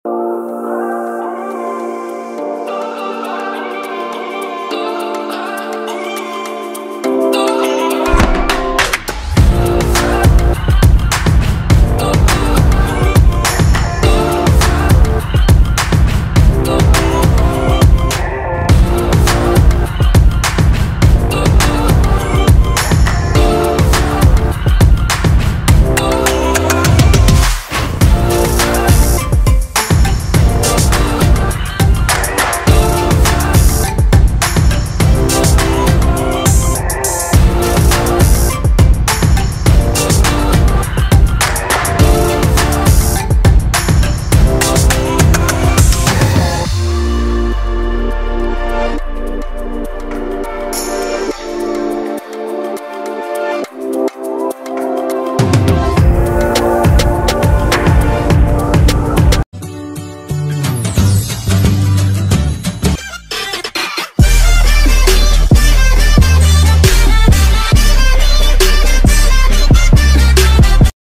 Healthy lunch / easy and healthy lunch / ready to eat salad recipe
1 Healthy, easy, and quick idea for lunch.You can put into kids lunch boxes too.this is very healthy recipe for weight loss too.I hope you will enjoy.